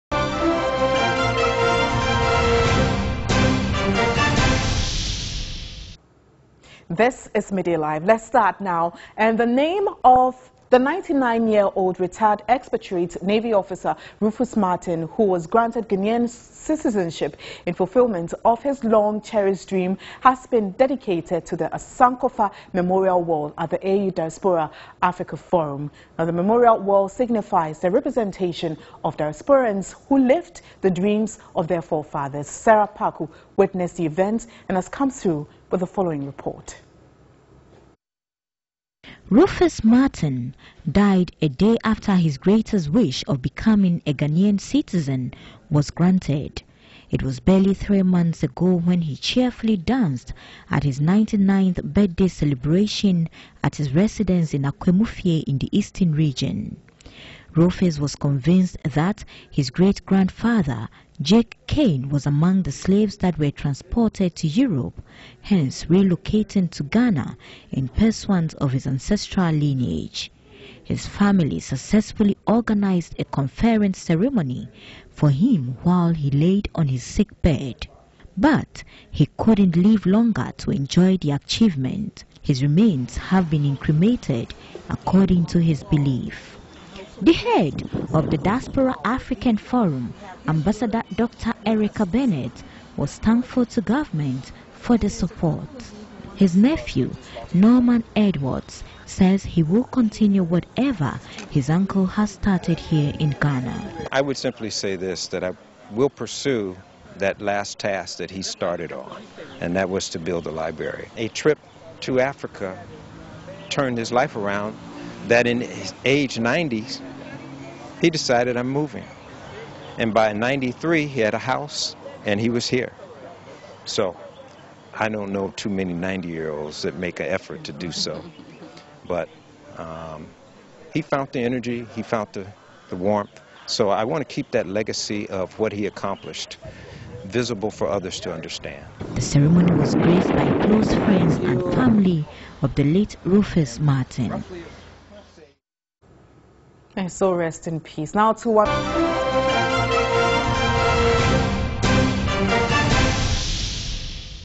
RUFUS ON SANKOFA MEMORIAL WALL
Name of 99 yr old late Rufus Martin honored on Sankofa memorial wall in Ghana.